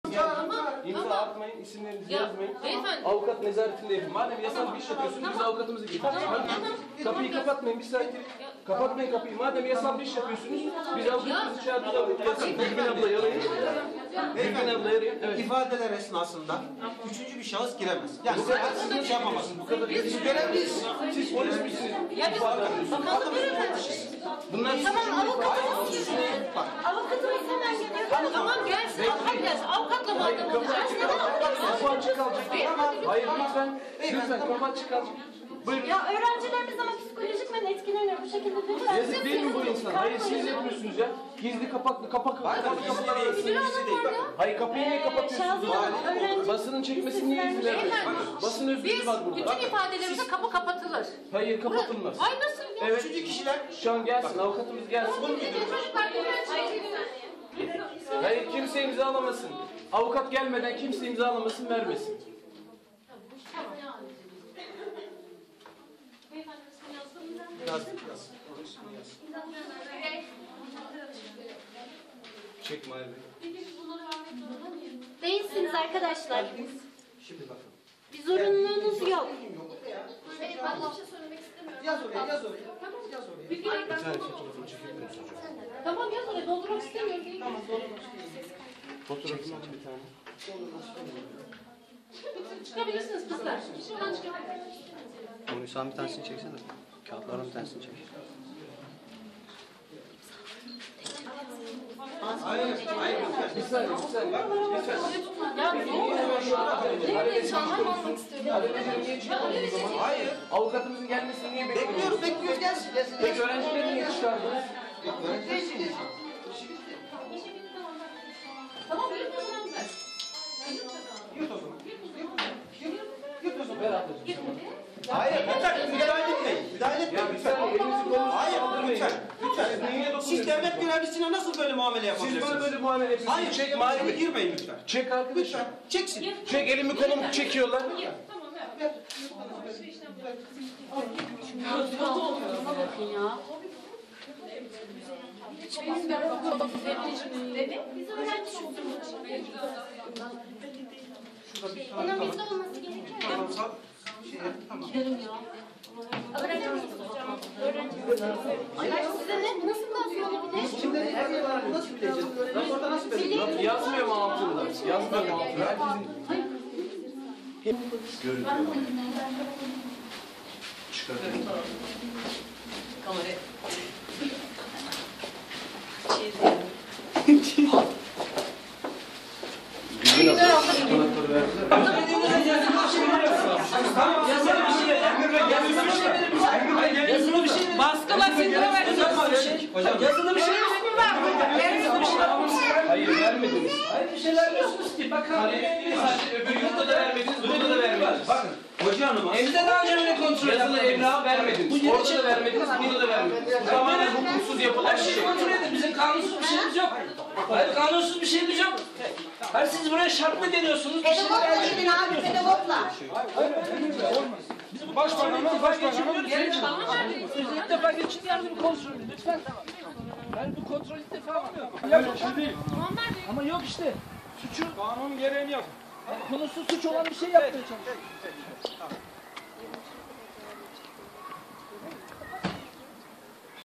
Çocukları sorgulayan müfettişler, gazetecilerin soruları üzerine polis çağırdı
Milli Eğitim Bakanlığı'nın (MEB) talimatıyla özel okul, yurt ve dershanelerde ikna odaları kuran müfettişler, gazetecilerin soruları üzerine polis çağırdı. Müfettişler, çocuklarının cep telefonlarını kapattırarak görüntü ve ses alınmasını de engelledi.